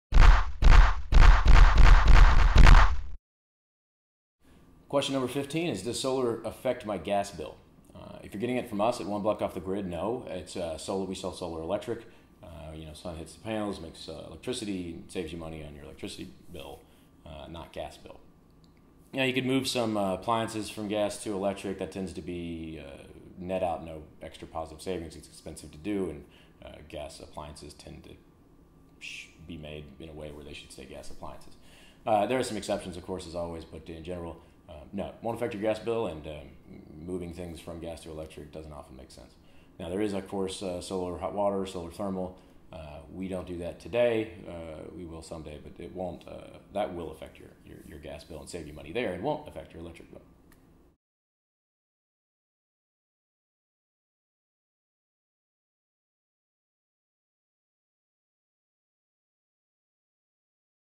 Will solar power change my gas bill or just the electric bill?
One Block Off the Grid's Dave Llorens answers the question: Will solar power change my gas bill or just the electric bill?